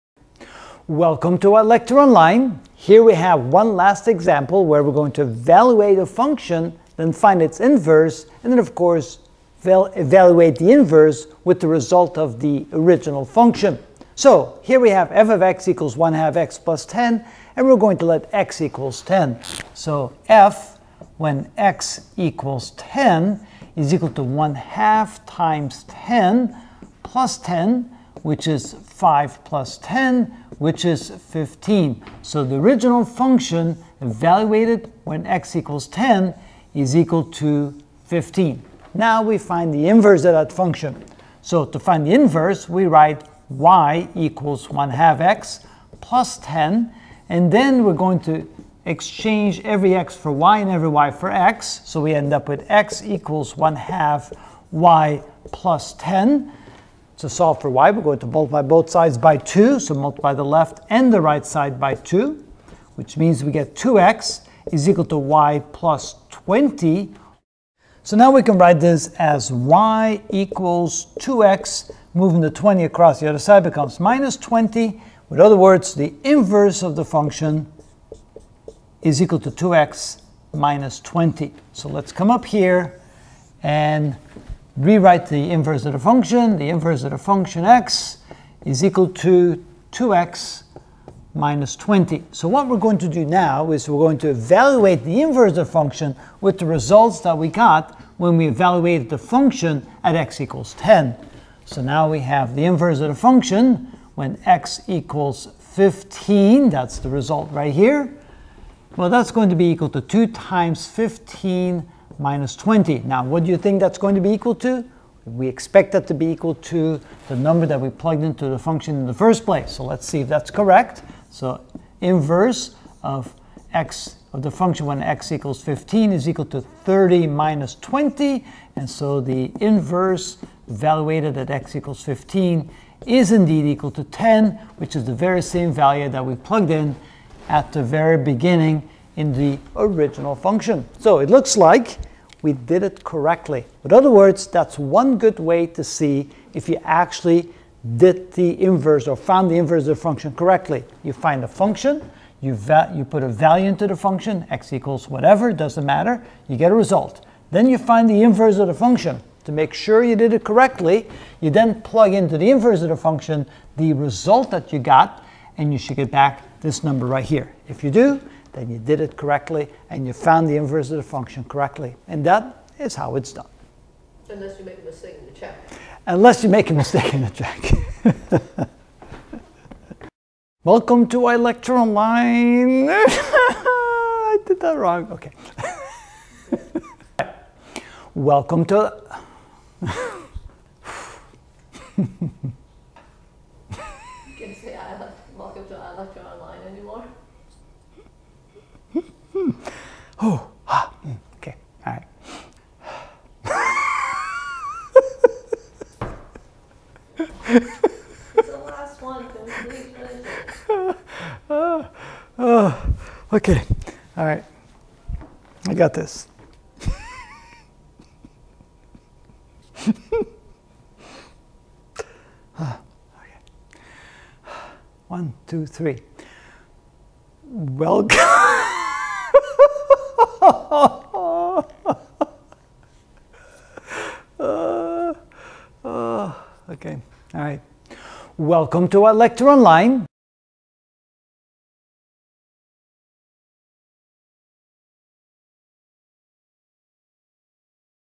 Algebra Ch 45: The Inverse of a Function (15 of 15) Evaluate the Function and its Inverse
We will find the inverse functions of:
f(x) =(2x+3)/(x+4)   f(x)=(-3/4)x+(9/4)   Example Set #2